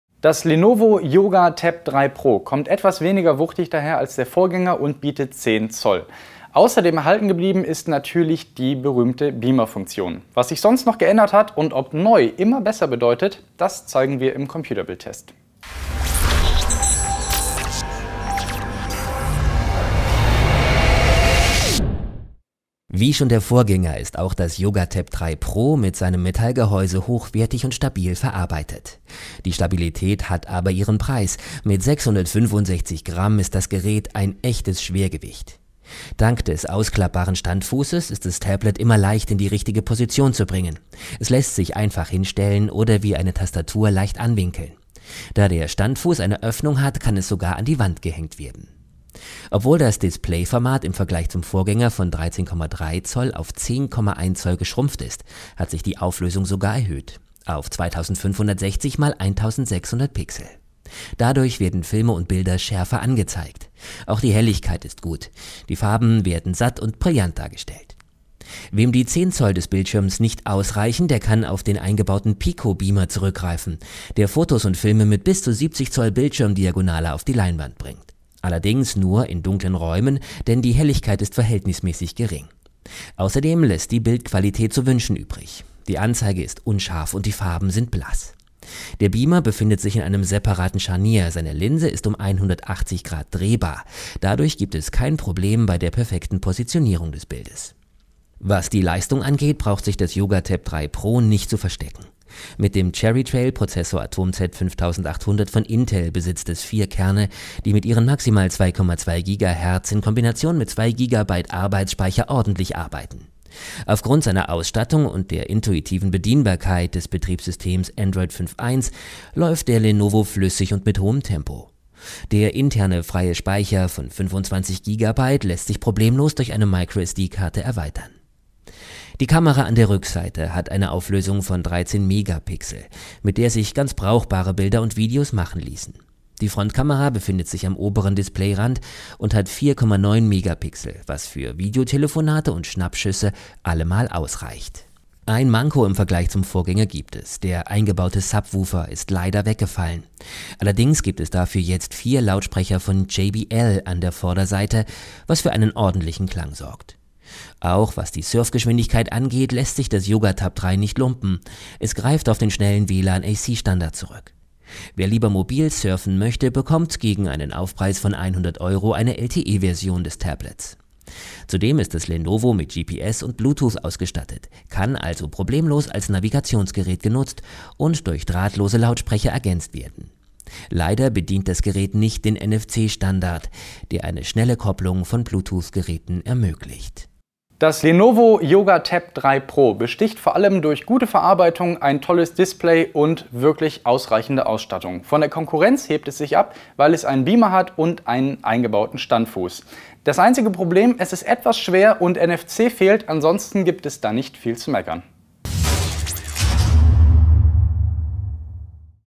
Lenovo Yoga Tab 3 Pro: Beamer-Tablet im Test
Das Lenovo Yoga Tab 3 Pro besitzt im Scharnier einen Beamer, der um 180 Grad drehbar ist. Wie ist die Bildqualität und was bietet das Gerät sonst?